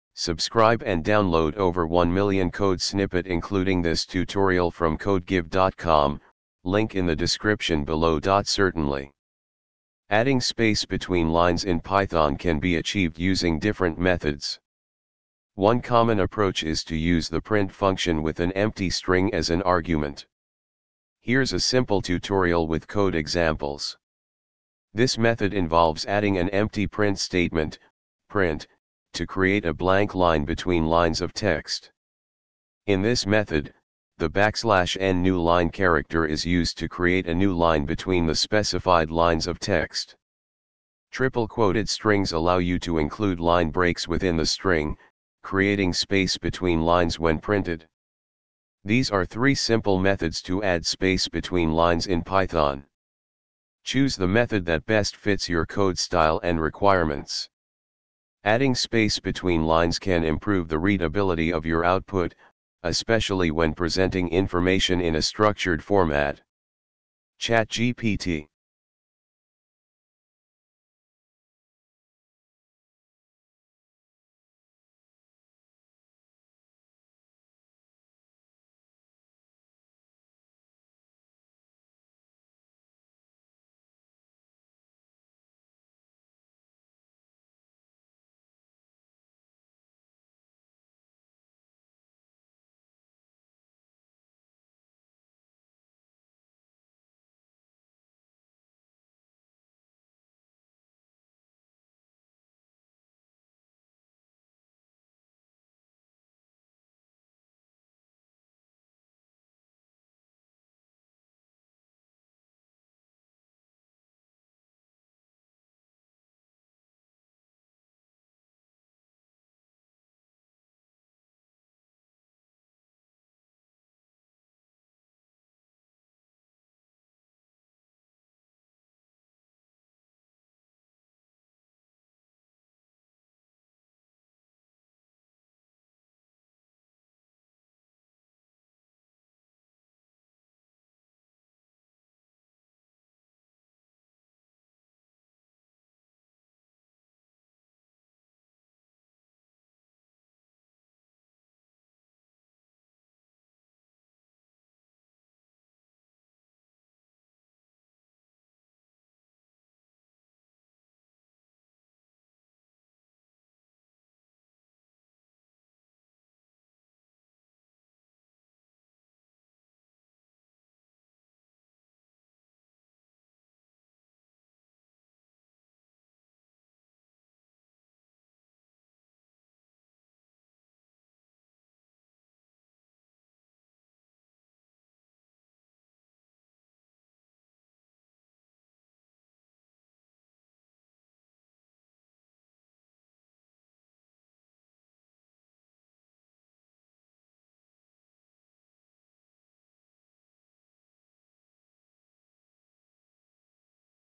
how to add space between lines in python
Certainly! Adding space between lines in Python can be achieved using different methods. One common approach is to use the print function with an empty string as an argument. Here's a simple tutorial with code examples:
This method involves adding an empty print statement (print("")) to create a blank line between lines of text.
In this method, the "\n" newline character is used to create a new line between the specified lines of text.
Triple-quoted strings allow you to include line breaks within the string, creating space between lines when printed.
These are three simple methods to add space between lines in Python. Choose the method that best fits your code style and requirements. Adding space between lines can improve the readability of your output, especially when presenting information in a structured format.
ChatGPT